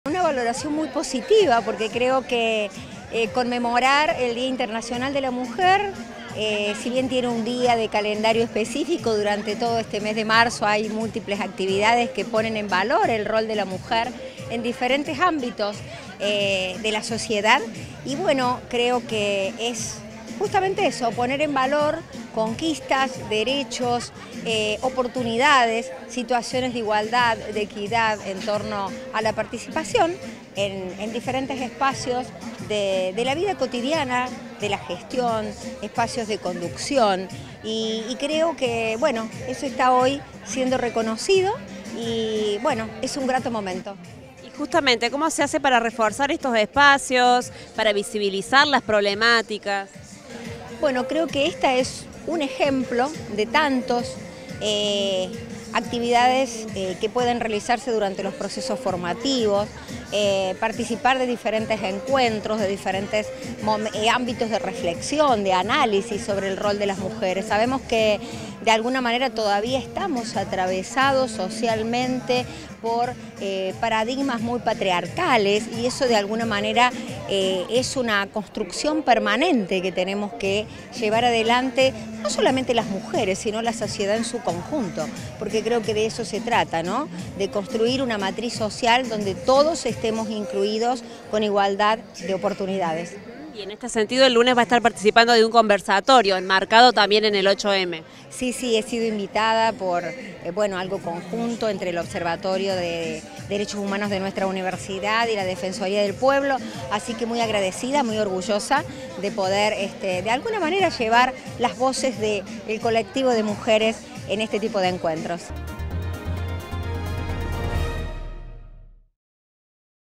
CON ACTIVIDADES CULTURALES Y DE REFLEXIÓN, SE CONMEMORÓ EN LA UNRC EL DÍA INTERNACIONAL DE LA MUJER
MARISA ROVERA – RECTORA
UniRio TV
Universidad Nacional de Río Cuarto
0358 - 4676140